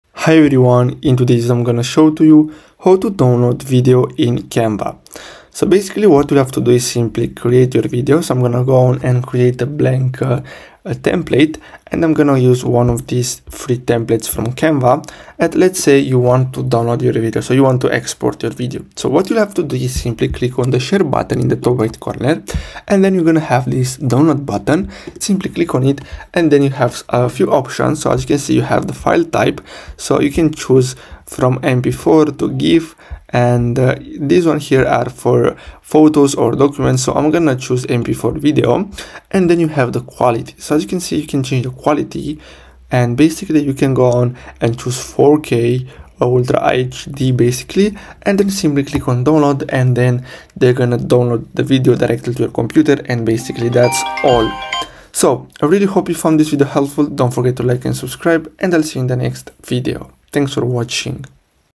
How to Download Video in Canva 2026 (Canva Tutorial)
Canva is a powerful online design platform that offers a variety of tools and features for creating impressive visual content. One of its useful features is the ability to download videos directly from the platform. To download a video in Canva, simply click on the “Download” button located on the top right corner of the screen.

This will bring up a menu with different file format options such as MP4, GIF, and video quality options ranging from low to high. Select your preferred format and quality, and click “Download” to start the process. Once the download is complete, you will have a high-quality video ready to use in your projects. With Canva’s seamless downloading process, creating and sharing professional-looking videos has never been easier.